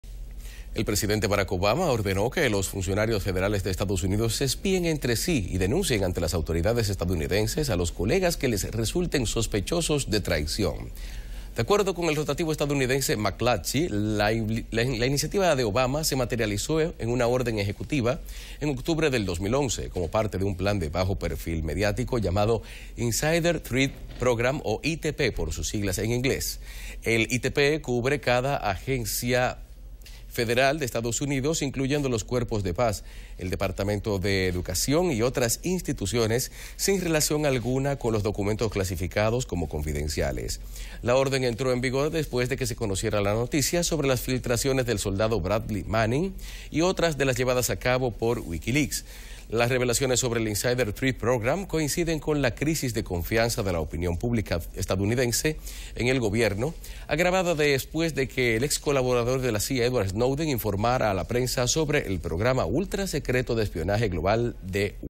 Obama ordenó a funcionarios espiarse entre sí y delatar a "Potenciales Traidores"
El presidente Barack Obama ordenó que los funcionarios federales de Estados Unidos se espíen entre sí y denuncien a las autoridades estadounidense a los colegas que les resulten sospechosos de traición